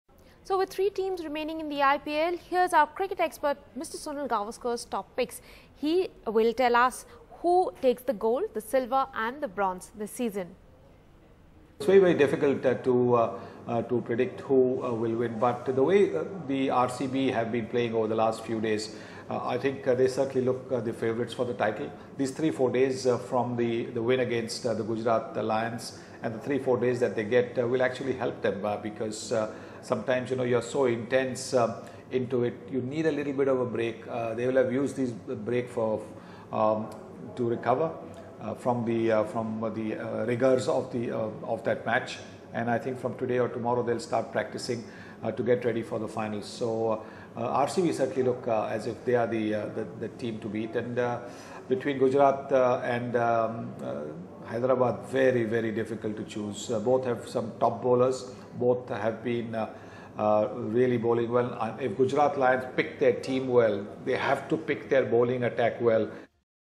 'Bowling big factor in Gujarat Lions vs Sunrisers Hyderabad IPL tie'
NDTV's cricket expert Sunil Gavaskar feels it is very difficult to pick a winner in the IPL match between Gujarat Lions and Sunrisers Hyderabad at Delhi's Feroz Shah Kotla on Friday night. The winning team will play Royal Challengers Bangalore in the final on Sunday. Gavaskar feels the result of the match will depend on the team with better bowlers.